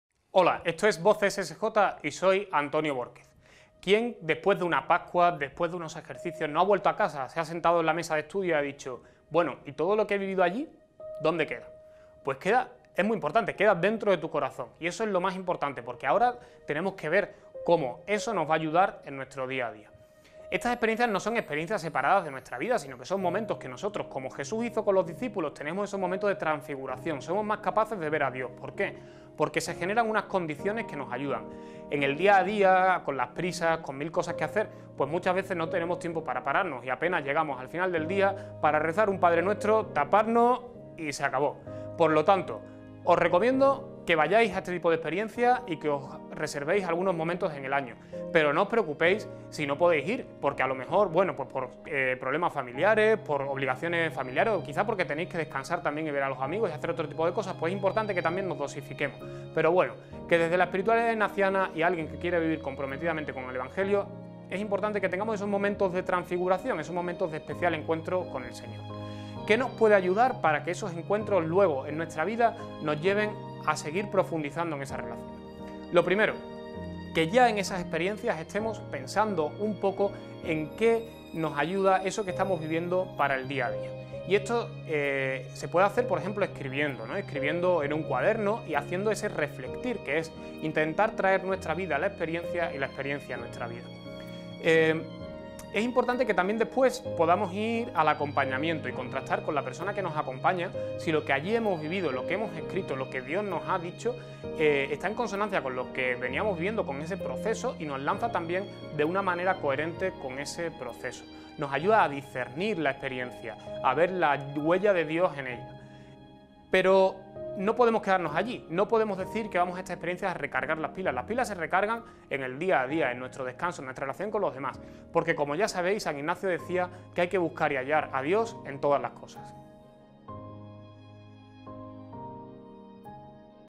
Experiencia espiritual | Antonio Bohórquez, SJ | Voces Esejota
Antonio Bohórquez Colombo sj nos da algunas claves para aplicar en nuestra vida después de una experiencia de pascua, campo de trabajo, peregrinación y que no quede todo en un bonito recuerdo.